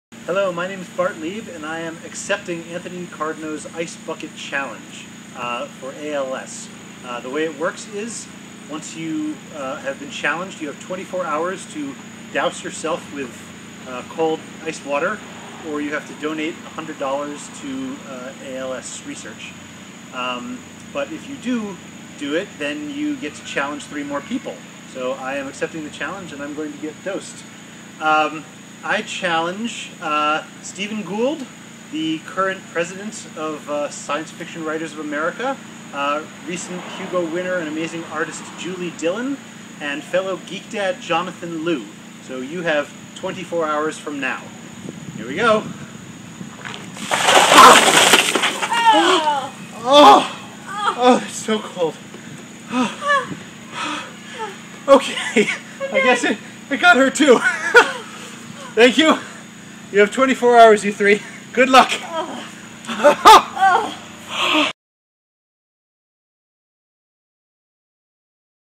Bart Leib's ALS Ice Bucket Challenge
I was challenged by Anthony Cardno to take the ALS Ice Bucket Challenge! Here's the results!